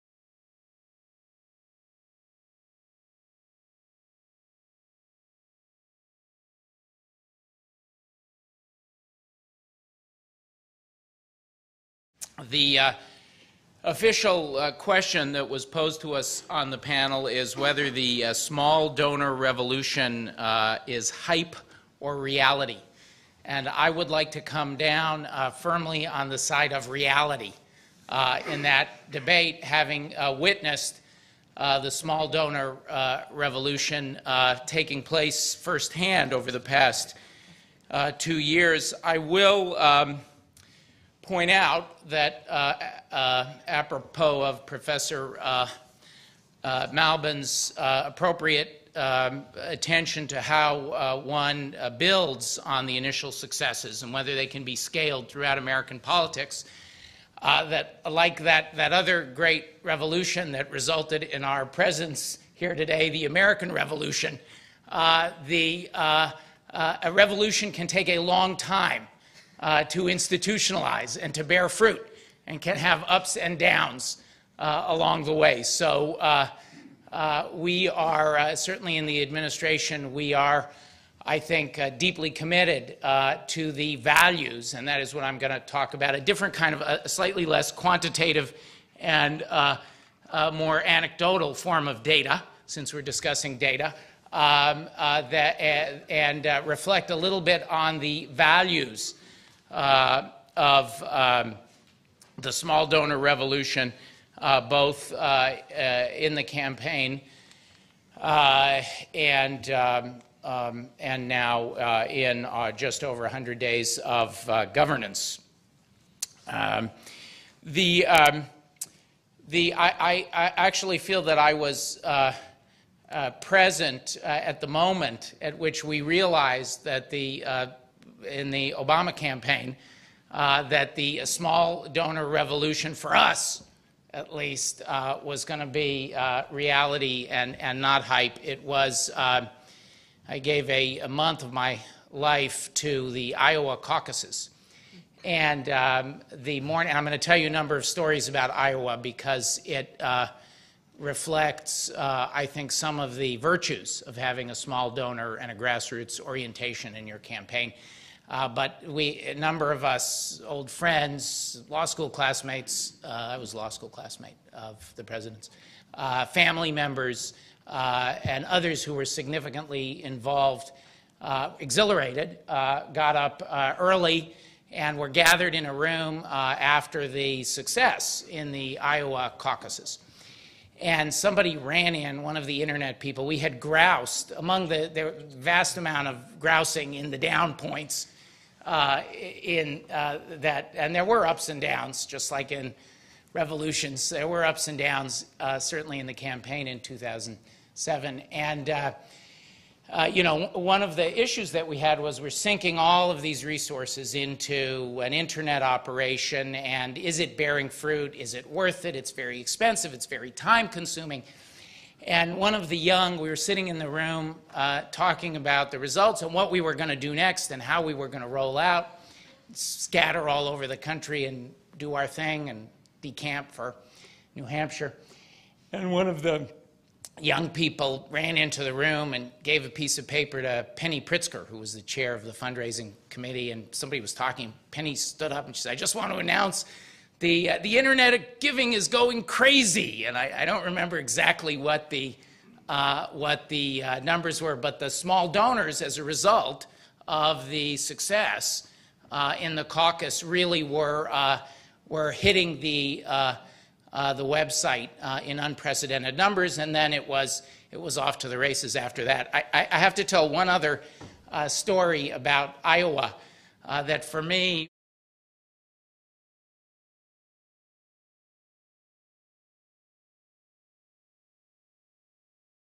Money in Politics 2009 | Norm Eisen (1/2)
Money in Politics 2009: New Horizons for Reform, a one-day conference held by the Brennan Center at the National Press Club. Norm Eisen (White House Chief Ethics Adviser) discusses his experience. Part one of two.

The Brennan Center invites speakers with a variety of viewpoints on a wide range of subjects in its events’ series. The views of the speakers are their own, and do not necessarily reflect the views of the Brennan Center.